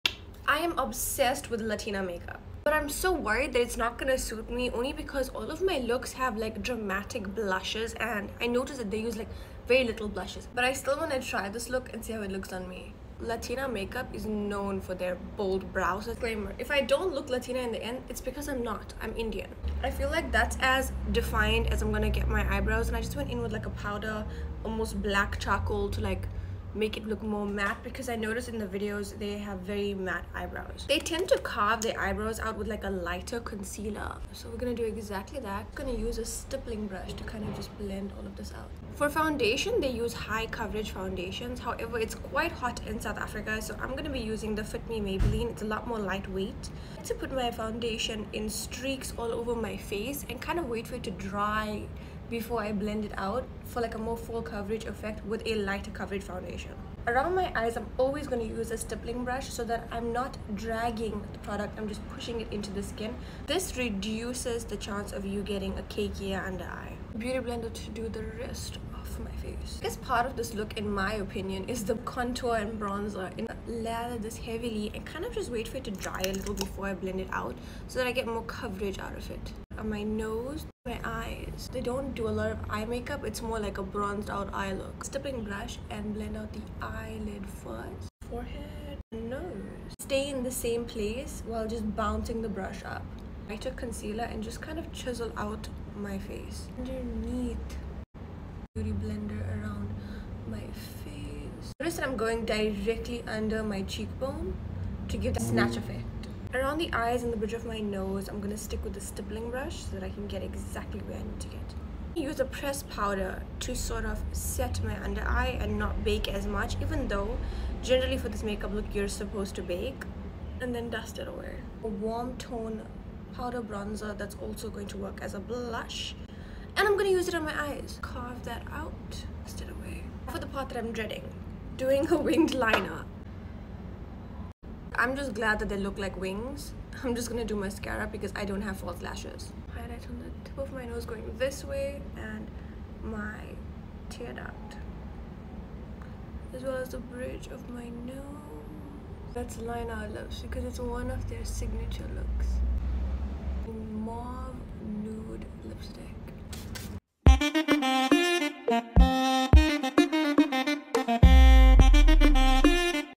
Latina Makeup on Indian Skin
Ethnicity really has nothing to do about it except for when it comes to Latina make up because oh my God what!!

I feel beautiful regardless of what make up I do but this it’s giving total bad vibes absolutely obsessed!